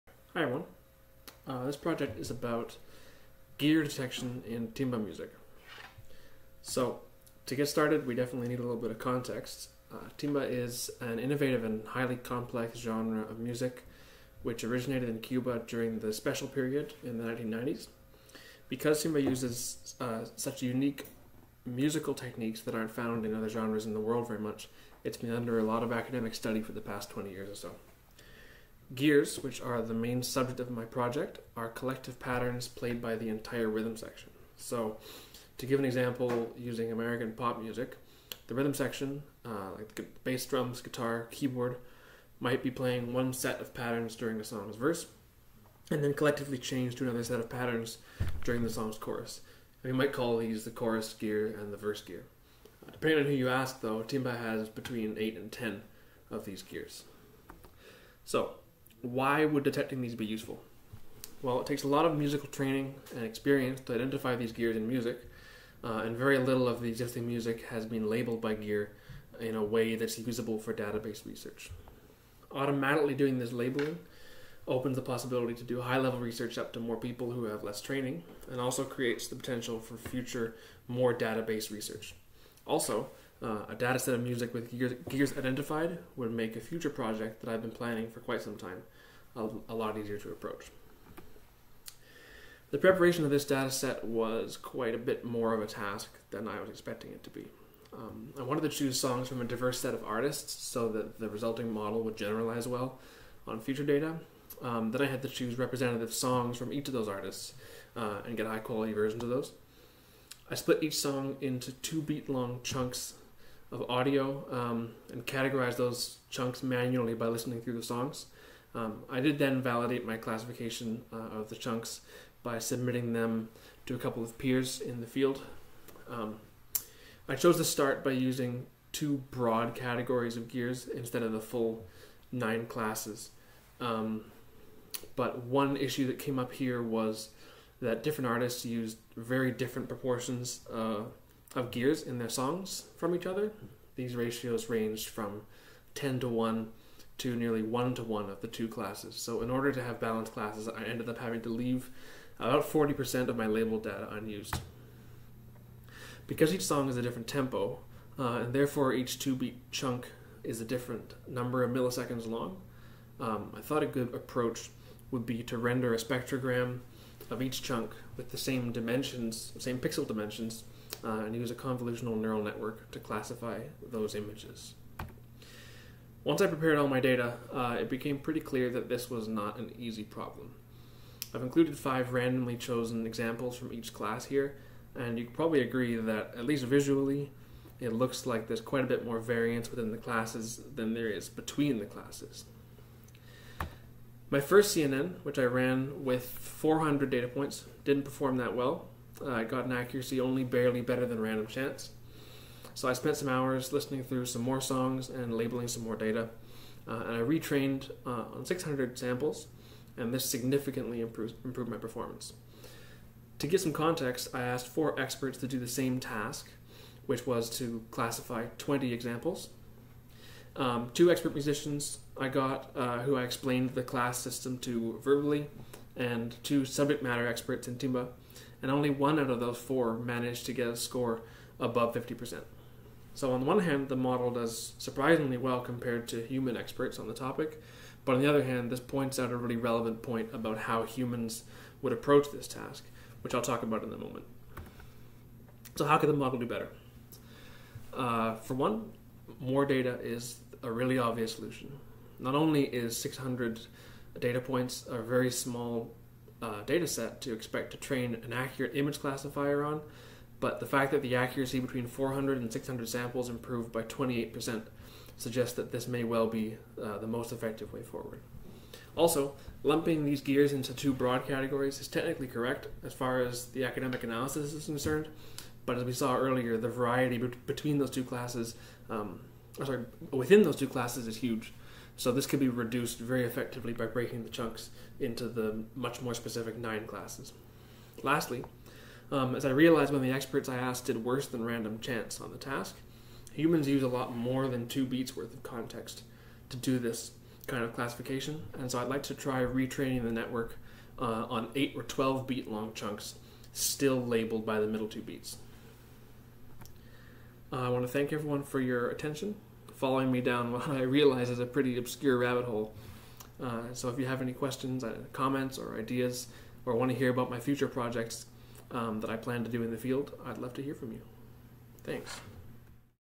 Timba Gear Detection
Short presentation about detection of rhythm section gears in timba using convolutional neural network image detection.